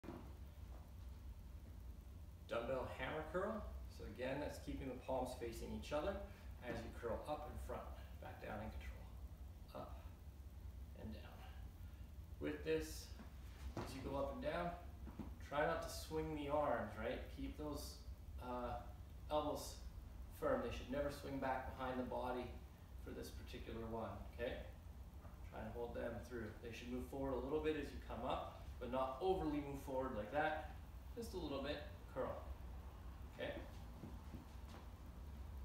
Arms Bicep Hammer Curl DB
keep elbows in front of body
elbows should not move backward as you curl the arm
hammer curl = neutral grip